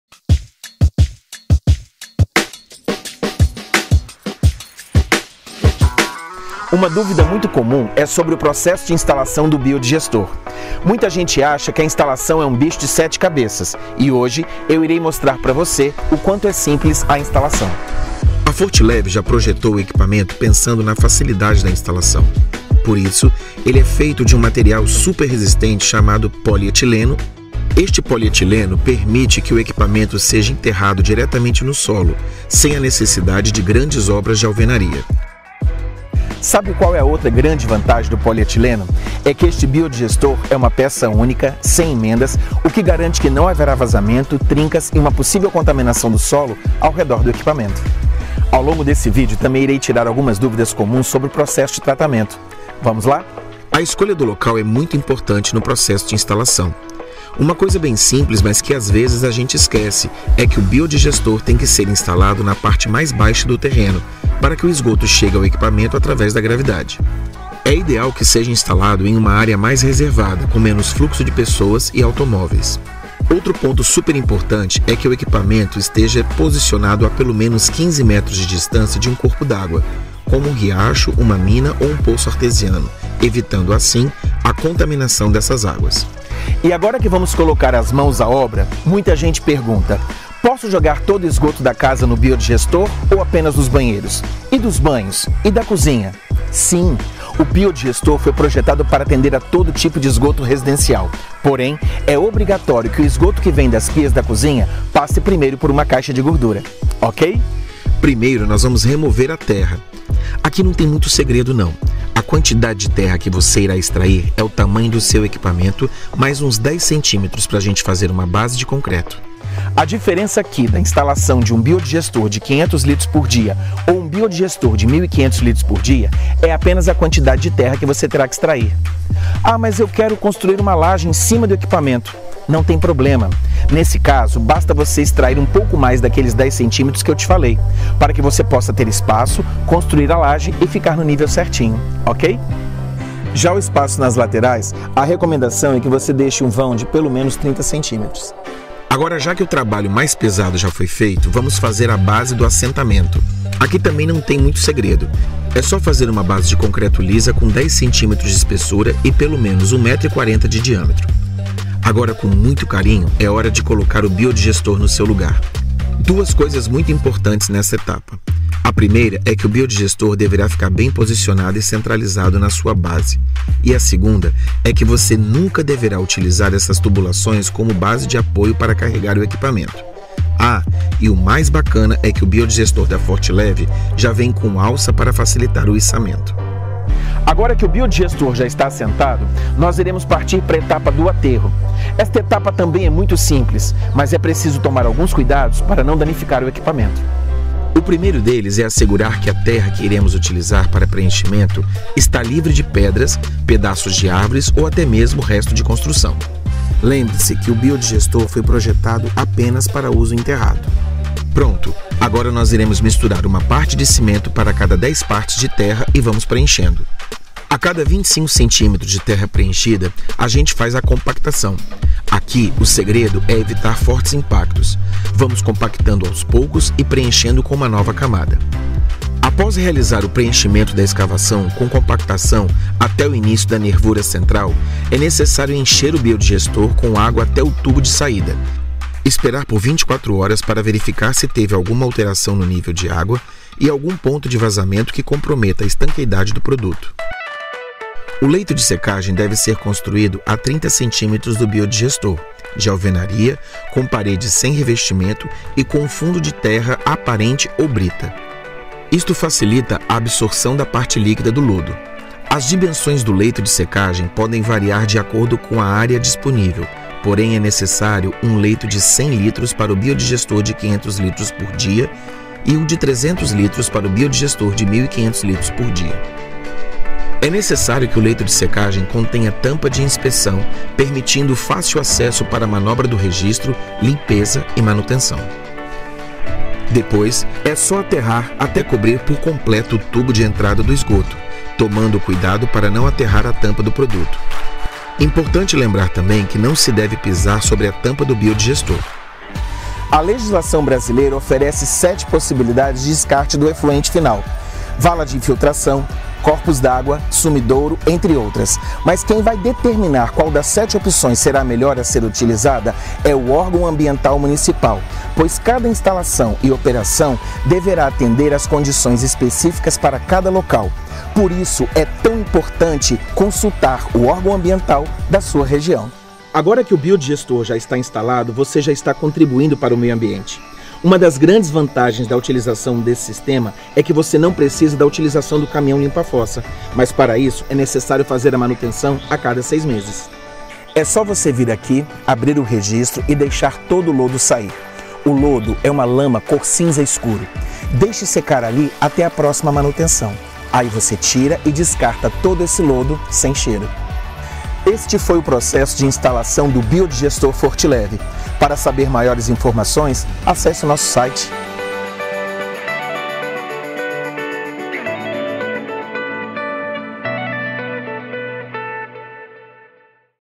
Manual Instalação Biodigestor Fortlev
Uma dúvida muito comum é sobre o processo de instalação do Biodigestor. Assista ao vídeo e conheça todos os passos para instalar o Biodigestor Fortlev.